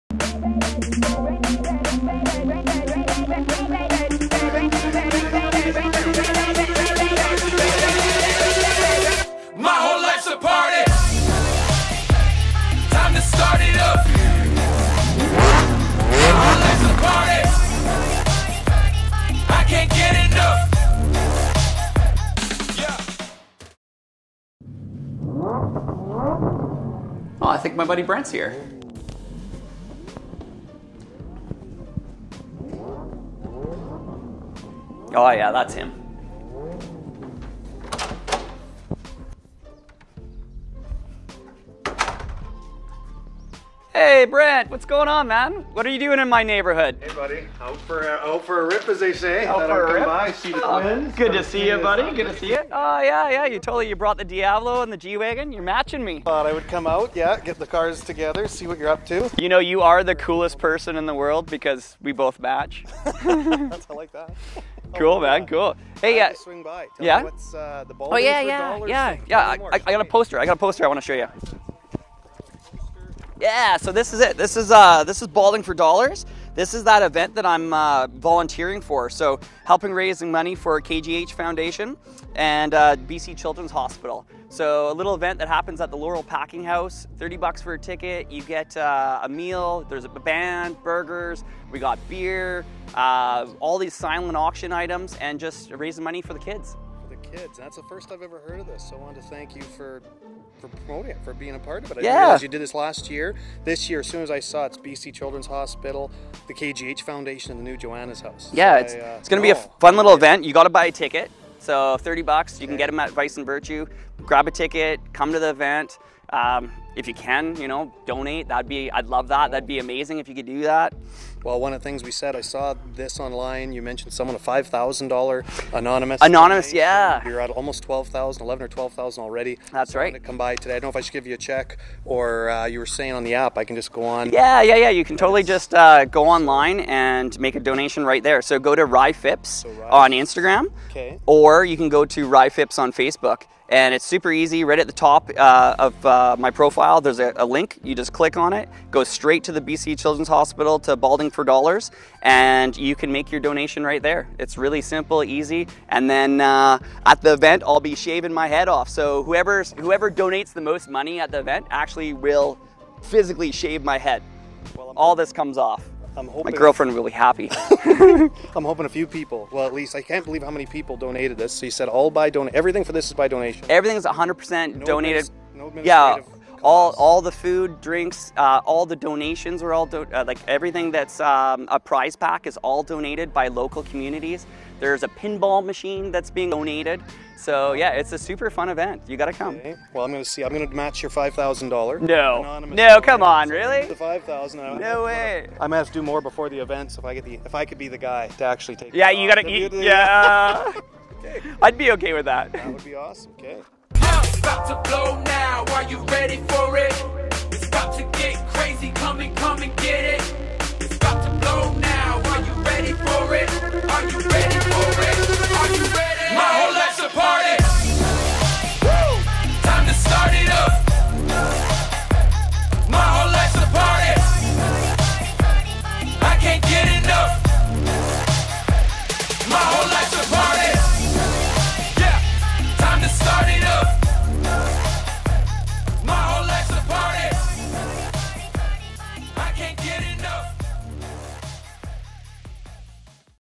2019 Balding for Dollars.
We had the pleasure of joining Ryan Fipke in raising awareness for the 2019 Balding for Dollars charity event.

Brent Marshall stopped by with his beautiful rides to get some info and make a very generous donation.

Be sure to get your tickets for the event from Vice and Virtue Brewing in Kelowna.

Huge thank you to all involved in this event and everyone who's donated.